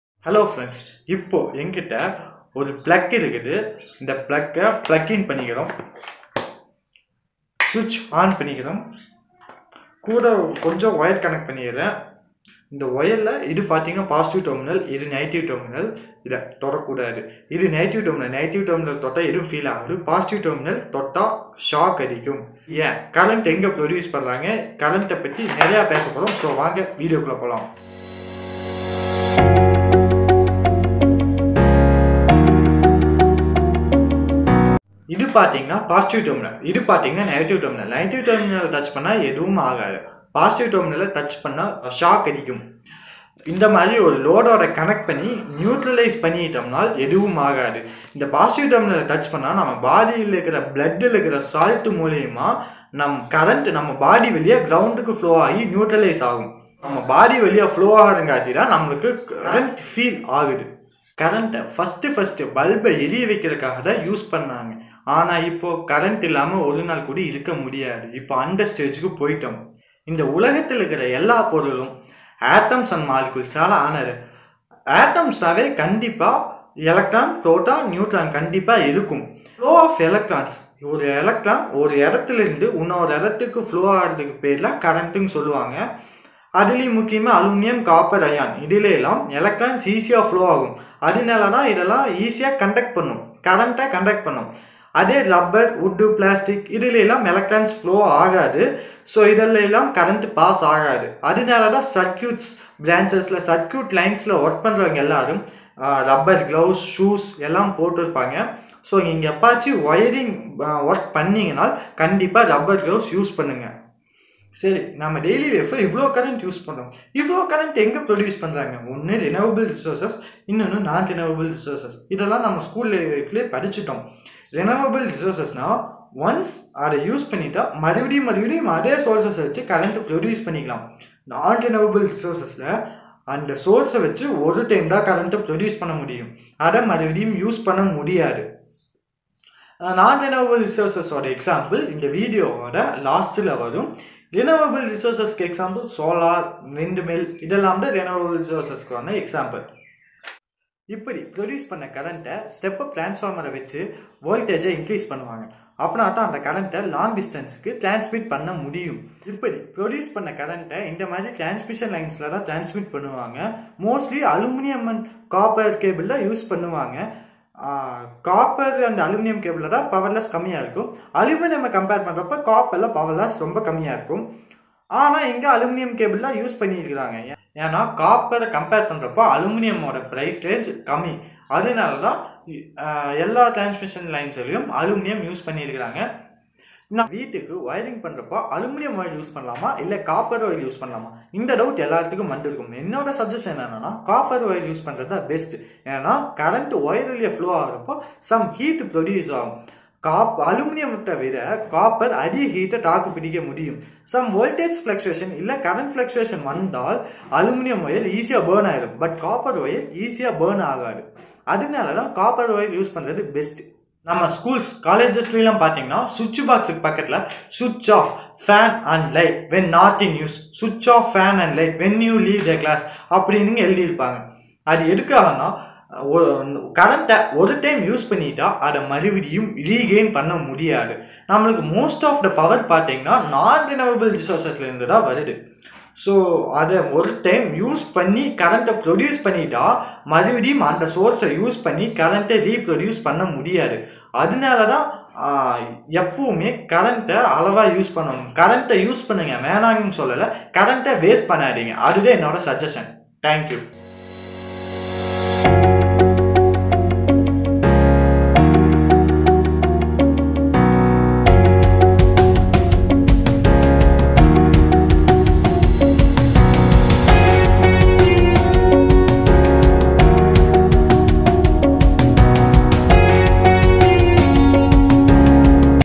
Where current is produced? | All details about current | Usage of current | Electronic spot
Current is a very important thing in our daily life. We need a current everyday. At first current is  used to blink a bulb.
Current is produced from rotating of turbines. Turbines are rotted based on the resources.
There are two types of resources :
1.RENEWABLE RESOURSES
2.NON RENEWABLE RESOURSES
Most of the current is produced using non renewable resources. RENEWABLE RESOURSES:
Wind mill, Solar energy, hydro energy, etc.
NON RENEWABLE RESOURSES:
Nuclear plant, Coal plant, etc.
This produced current voltage is increased by step up transformer and transmitted through aluminum cable.
Copper and aluminum is the best material to transmit a current. For house hold usage copper wire is the best option.

USE A CURRENT,BUT DON'T WASTE A CURRENT.